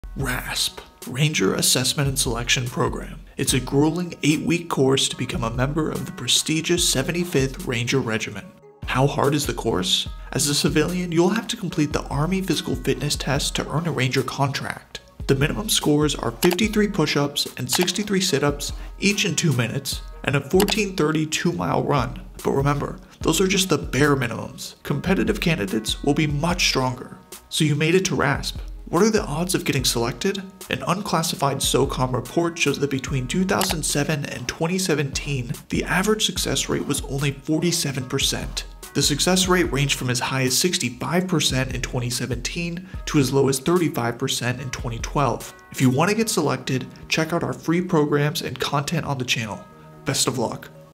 HOW HARD IS 75th RANGER REGIMENT? | RASP
Regardless of which Special Operations Forces career a trainee aspires to join, whether it’s Army Ranger, Navy SEAL, Navy SWCC, Air Force Pararescue, or Army Green Beret, they will have to complete the military entrance requirements which include push ups and sit ups, and then in selection they’ll also have a pull up or chin up test.